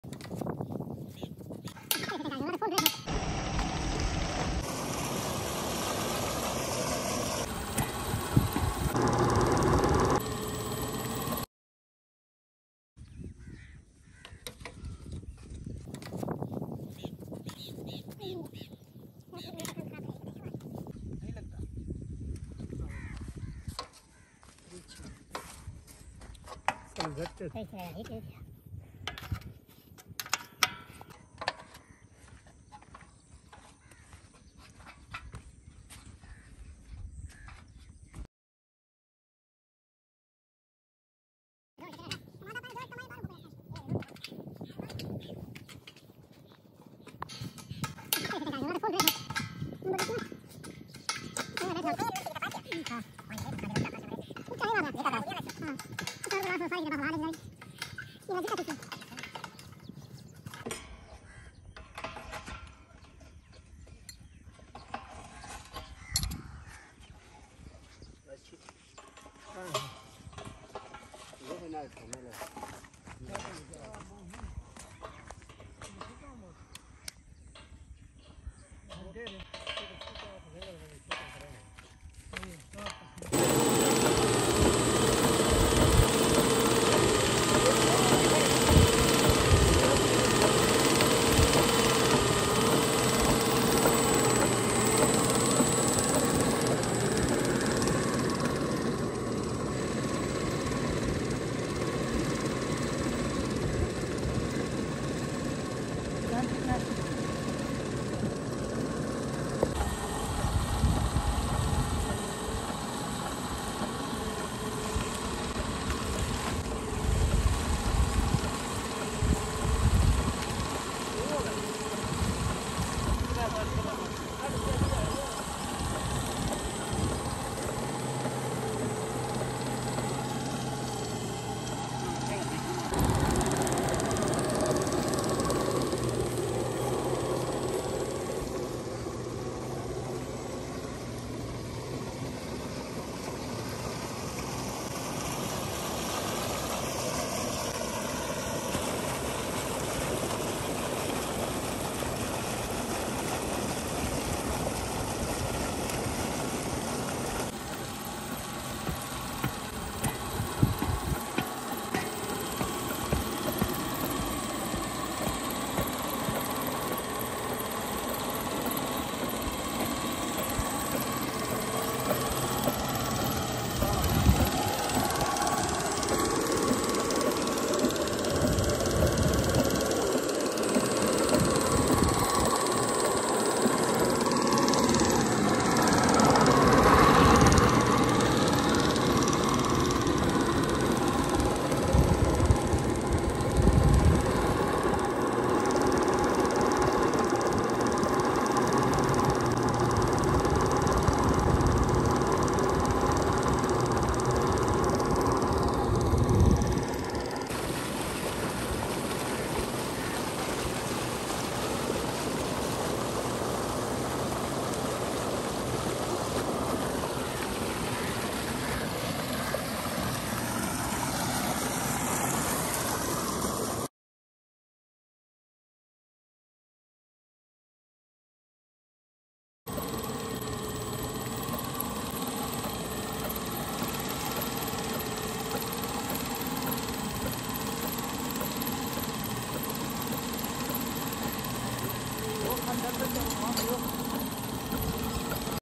How to install Tube Well System in Punjab
The best solar tube well system in Punjab. Poor farmer working hard to setup solar panel system on the bank of river.
solar power